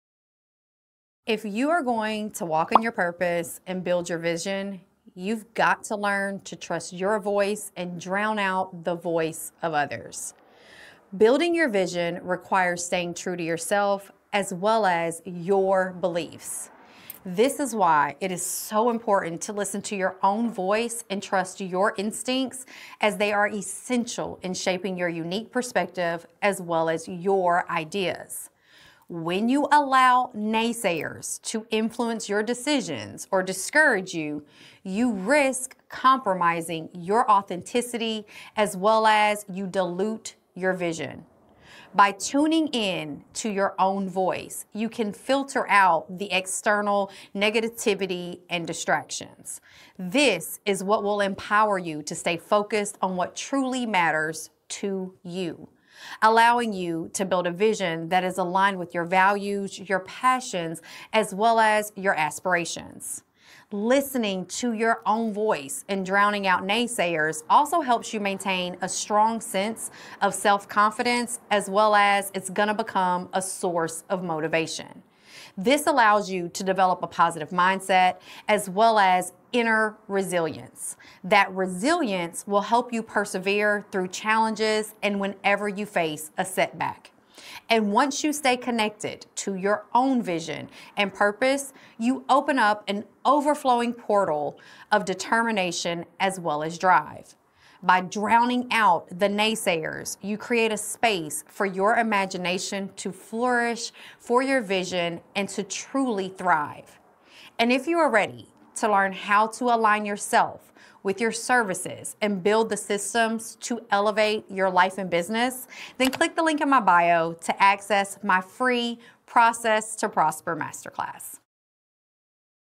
Listen to YOU
If you are going to walk in your purpose and build your vision….you've got to learn to trust your voice and drown out the voice of others.

I go deeper in the week's